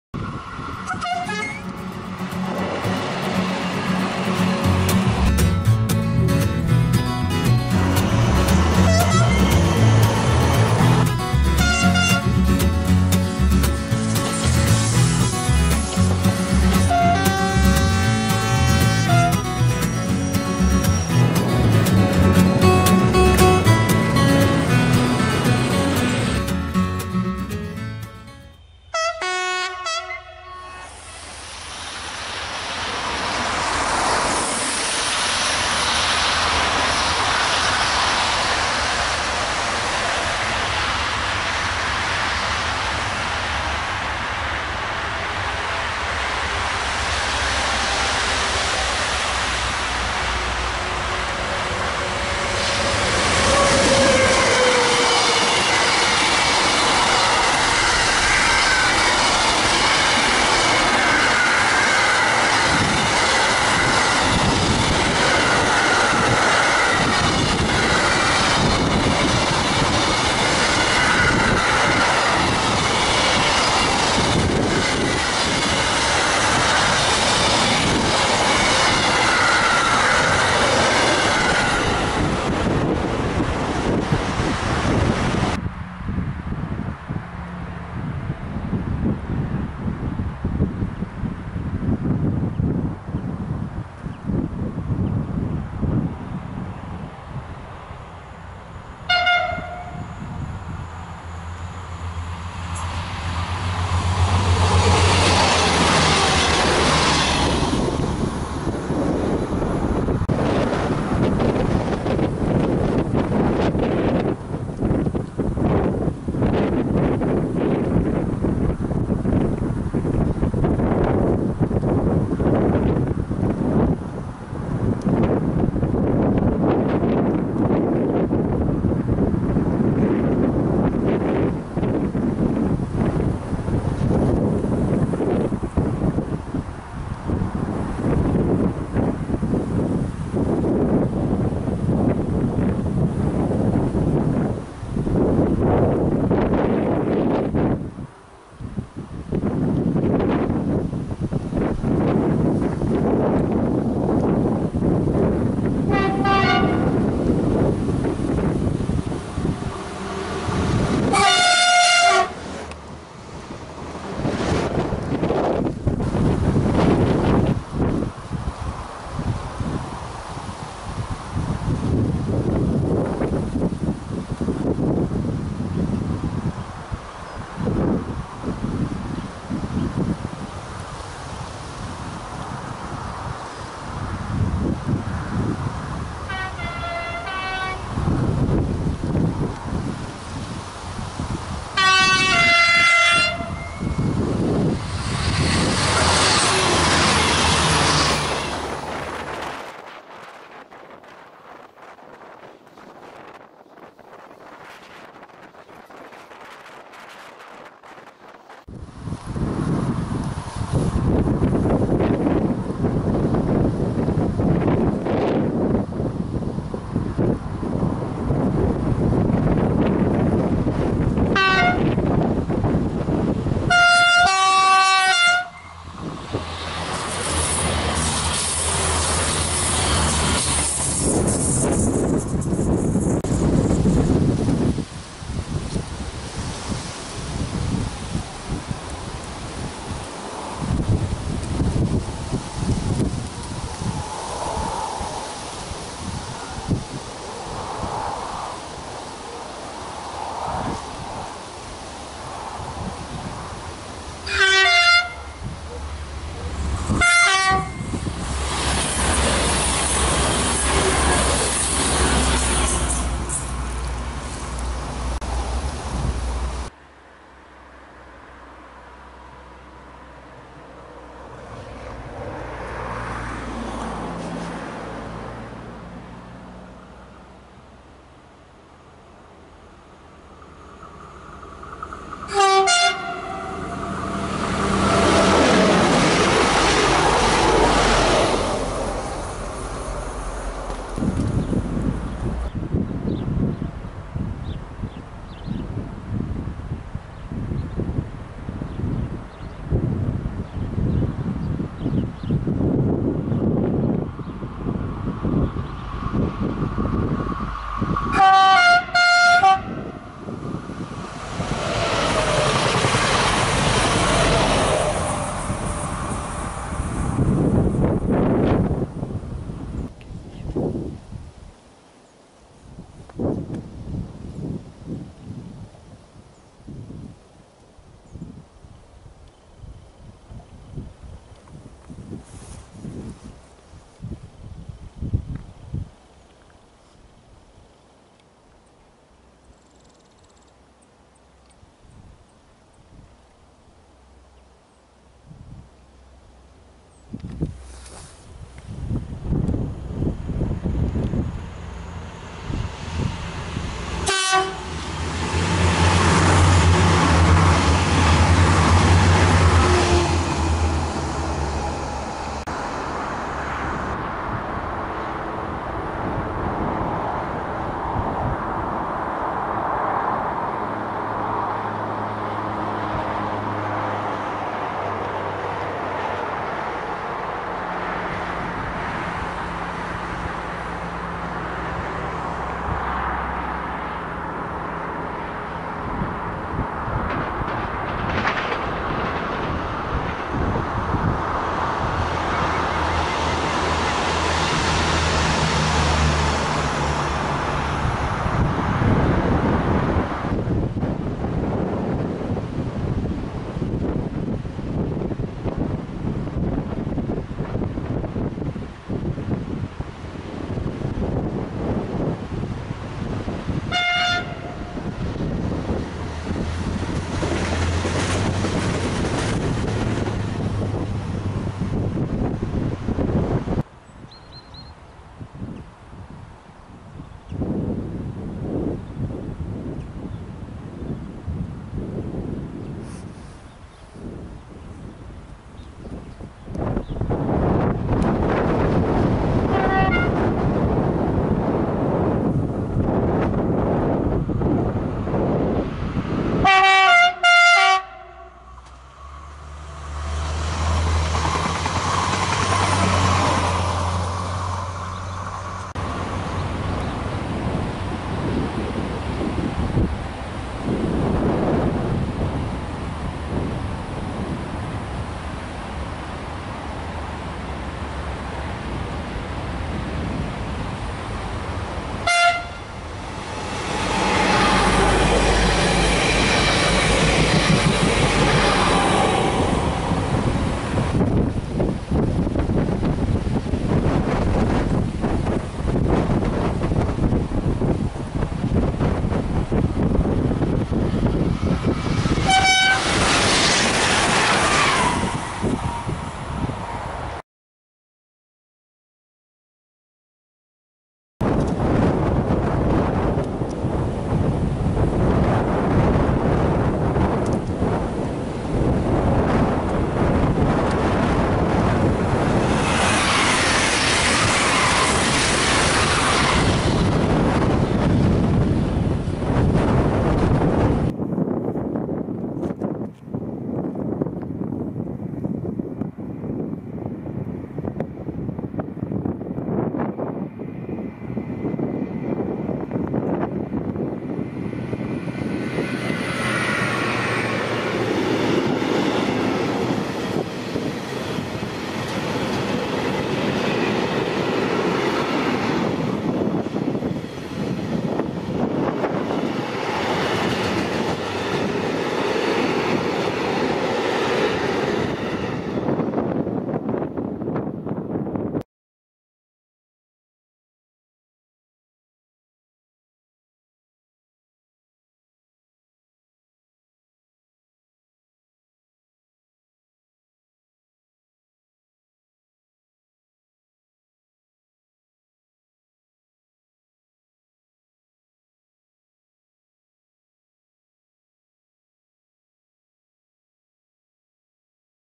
Les trains du week-end de pâques 2018 Mars-Avril 2018 BB 26000 BB 75000
Bonjour à tous!! Pour cette nouvelle vidéo, je vous propose une petite compilation de tous les trains que j'ai pu filmer pendant le week-end de pâques 2018. Tout d'abord le vendredi 30 mars 2018 vers 17h à Livron avec un seul fret en 35 minutes... puis le samedi 31 mars, je suis allé spotter une après-midi avec les 3 ferrovipathes à La Bâtie Neuve. Nous avons vu des TER assurés en X TER et AGC du TER PACA et les mécanos nous ont offerts de jolis sifflets!! Merci à eux!! On continu le lendemain, dimanche 1 avril, sur les rives du Lac de Serre Ponçon. Au programme, toujours des TER en X TER et AGC avec des sifflets et le dernier "train des neiges" de la saison 2017/2018. Pour terminer, le lendemain lundi 2 avril à Livron avec 3 TER assurés par des BGC et un fret tracté par la BB 75063 louée par VFLI à la SNCF. Merci aux mécanos pour les sifflets et bon visionnage!!!